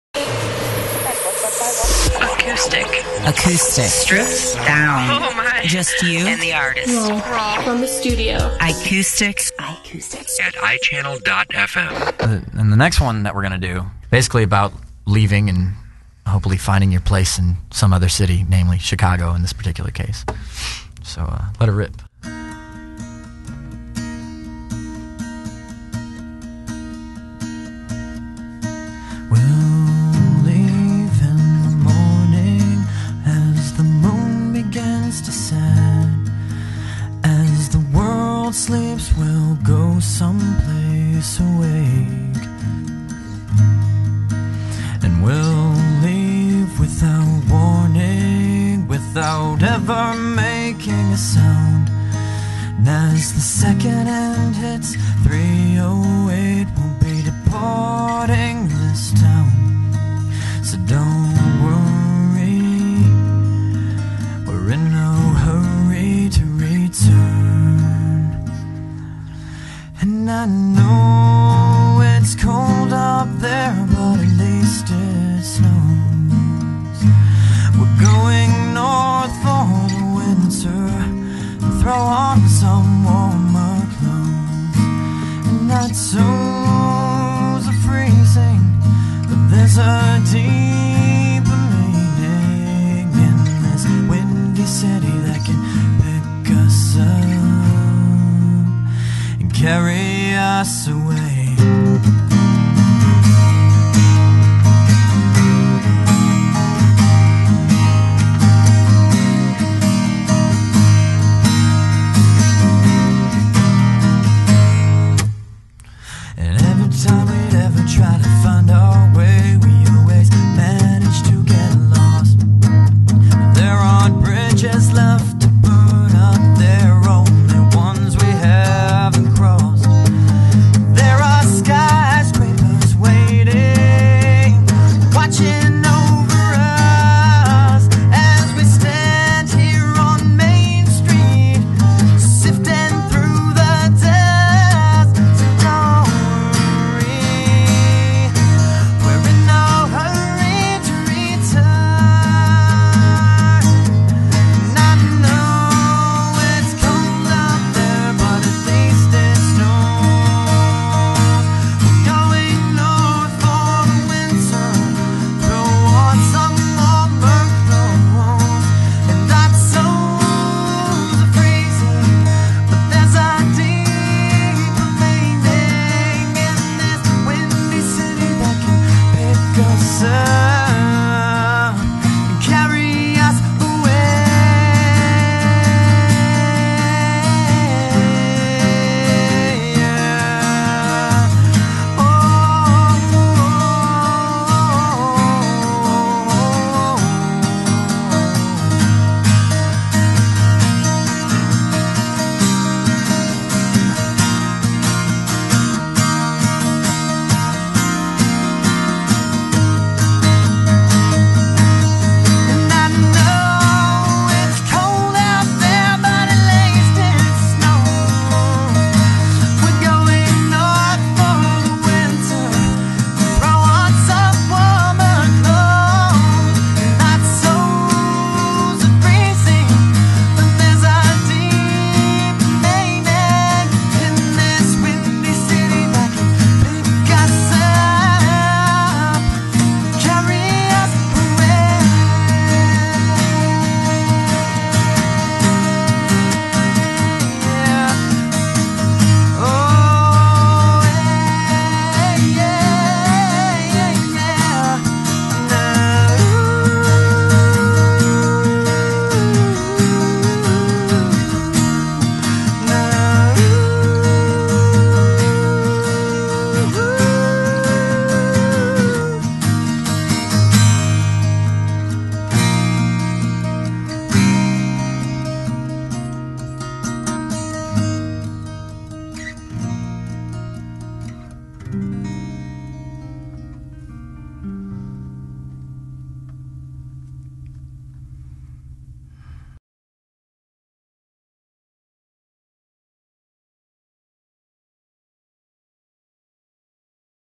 iChannel's iCoustix Session with Everlight 2 of 3
Everlight is the band of the week for iCoustic and they took full advantage of the moment.  Check out the story within the music and the lyrics that take you through the smoothness of the delivery. First To Hear, First To Know® Here is song 2 of 3 of a raw and stripped down iCoustix Session of Everlight from the studios and the iChannel HD Radio network.